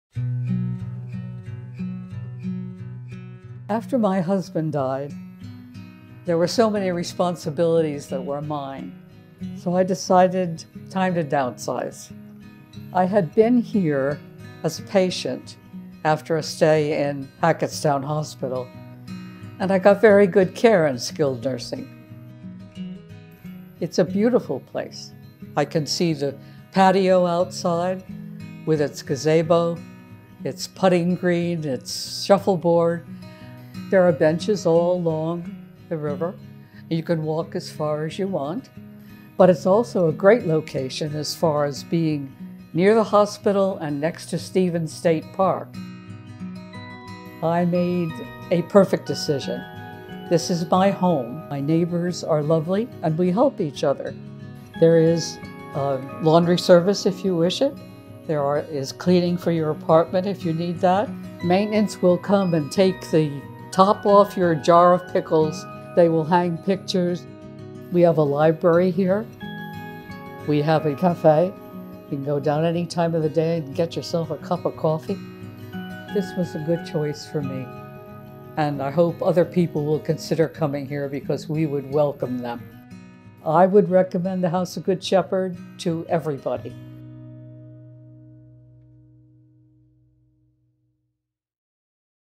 House of the Good Shepherd Resident Story- Meet Jean
Jean a long time resident of the House of the Good Shepherd shares what prompted her move to Independent Living and why she chose The House as her home